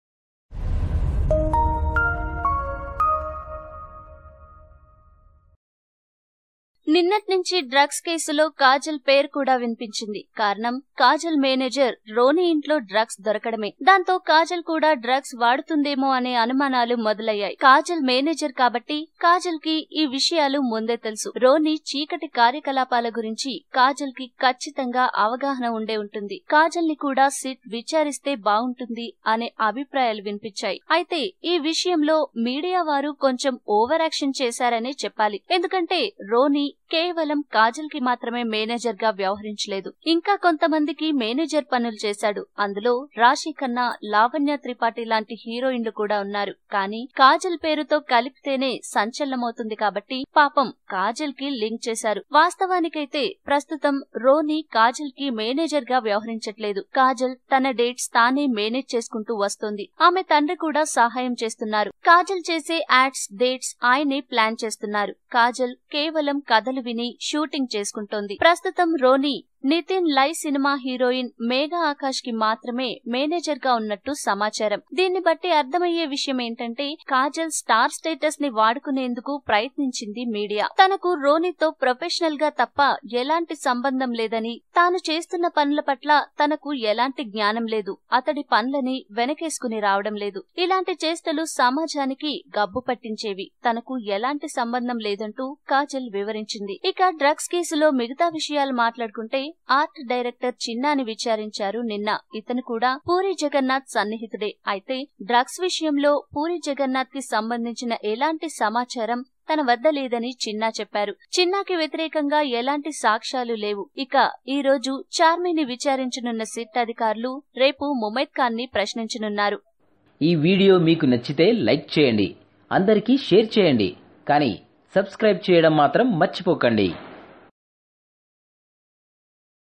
Kajal Aggarwal Unnecessarily Linked in Drugs Scandal | కాజల్ ని అనవసరంగా డ్రగ్ కేసులో ఇరికించారు
Kajal Aggarwal Unnecessarily Linked in Drugs Scandal. Check out the full details in this video on కాజల్ ని అనవసరంగా డ్రగ్ కేసులో ఇరికించారు on Vtube Telugu.